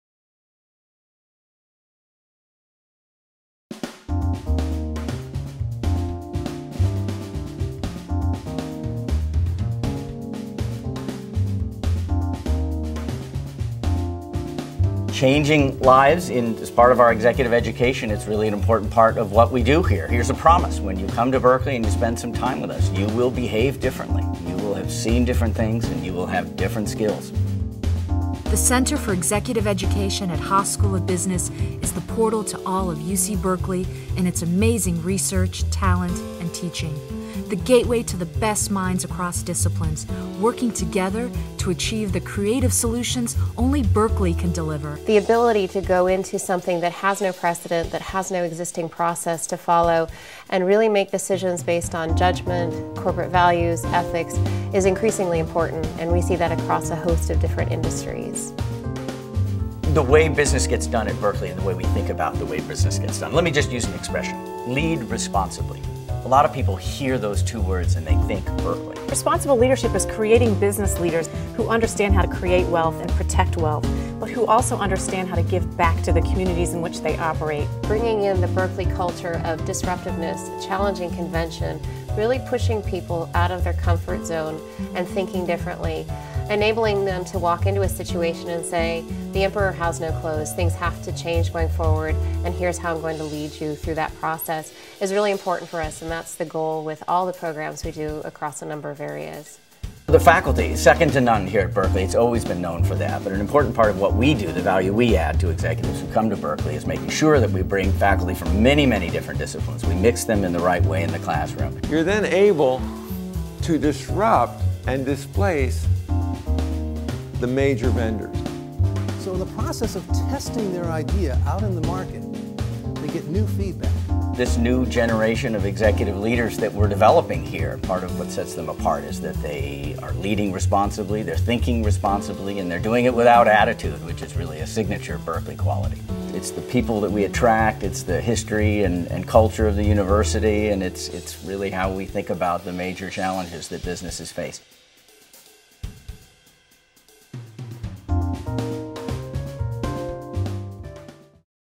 Center for Executive Education - Welcome
Dean Rich Lyons, Assistant Dean Whitney Hischier, and Haas Adjunct Prof. Kellie McElhaney introduce the UC Berkeley Center for Executive Education at the Haas School of Business and its approaches, advantages, and goals for participants.